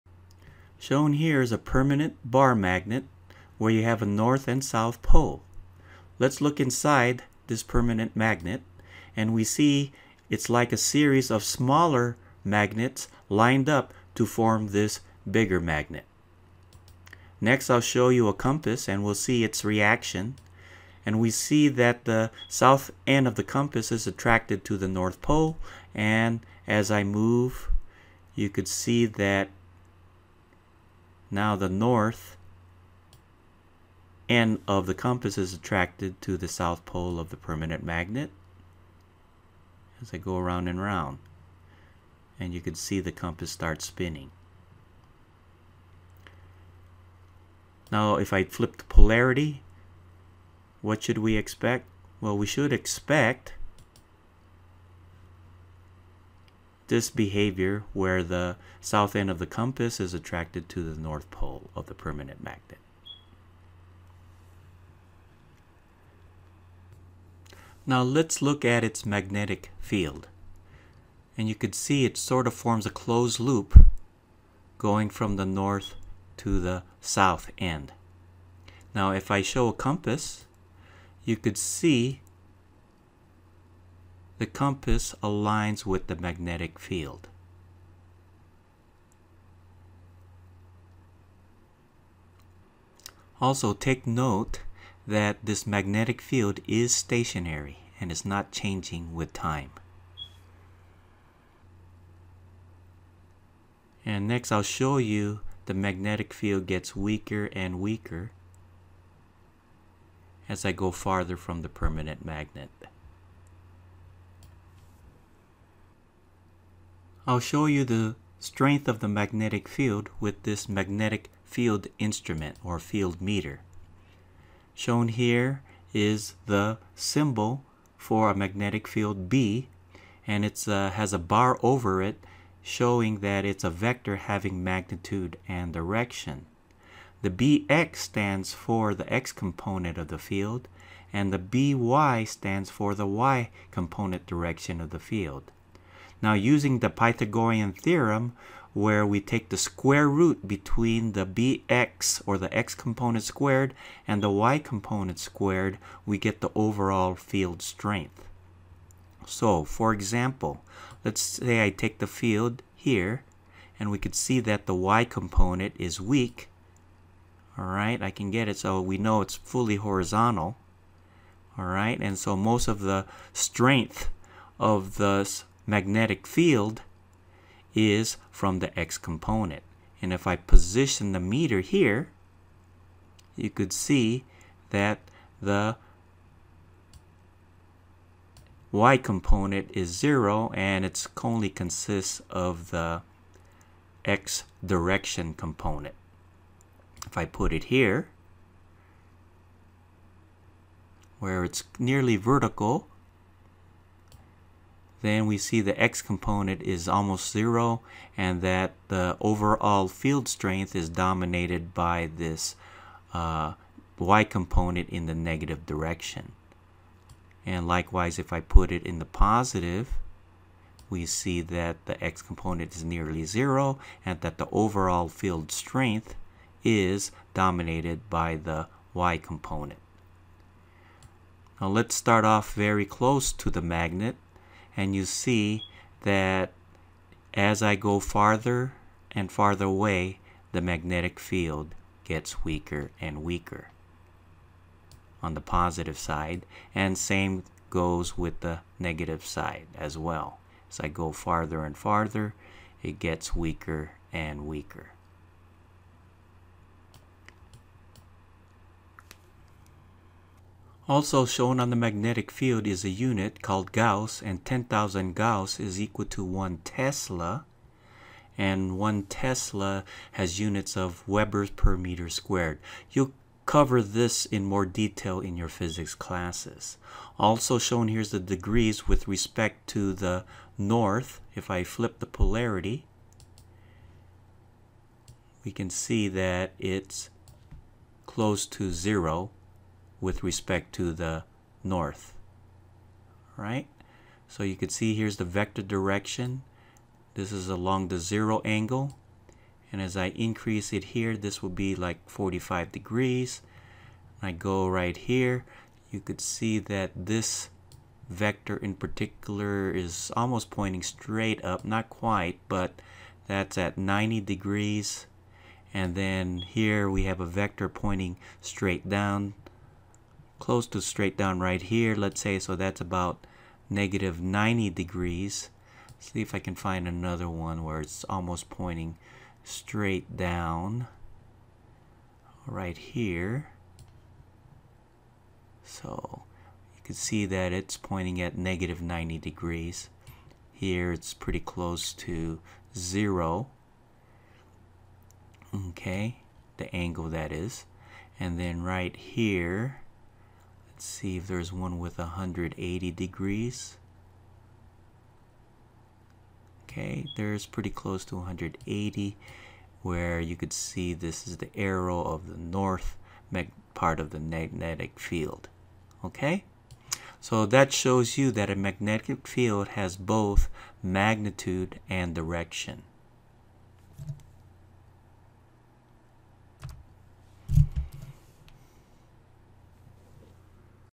Phet Simulation: Faraday's Lab on the Bar Magnet
Using the PHET simulation the basics on the bar magnet are presented.